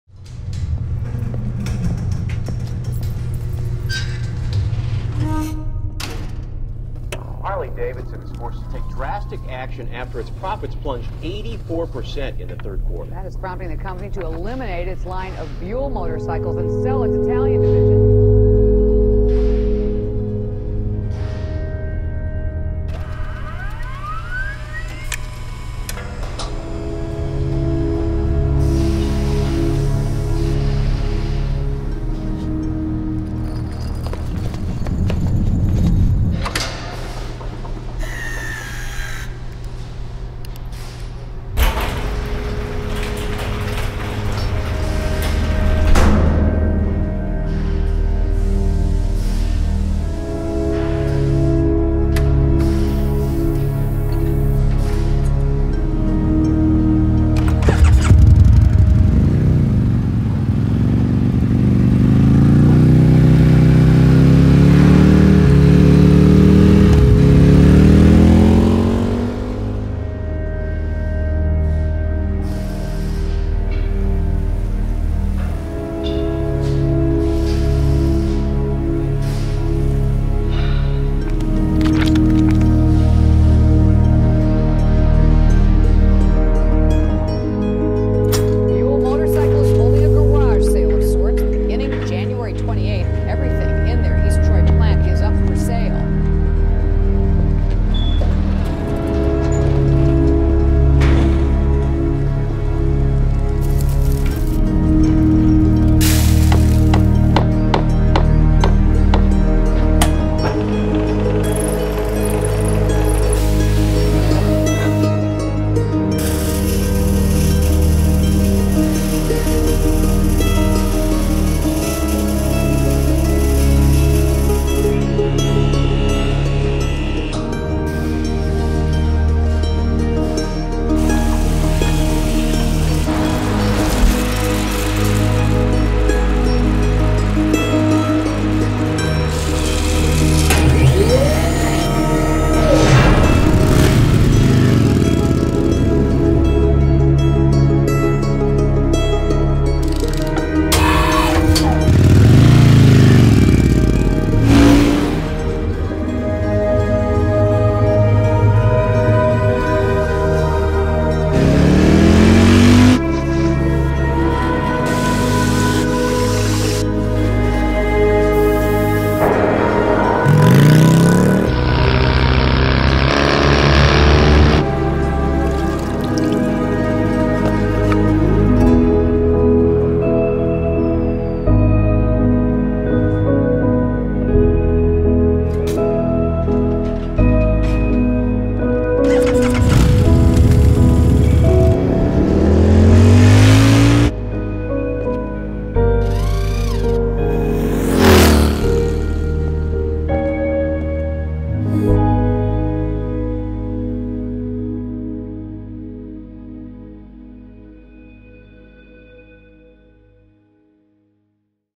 Ronin Motorcycle | A Brand Film | Shot on RED
Futuristic Films crafts a brand film for Ronin Motorworks—highlighting the intricate manufacturing process for their limited production motorcycle. Unlike a typical custom motorcycle, the team at Ronin Motorworks combines old world craftsmanship and modern manufacturing methods to create a production-grade motorcycle like no other. In their own words, “The art of making is not a clean, perfect process. It’s a battle. It’s controlled chaos.” Modeled after the story of the 47 Ronin, only 47 motorcycles will be created before production ceases forever. Nearly 6 years after it’s inception, the team at Ronin has begun delivering the first bikes.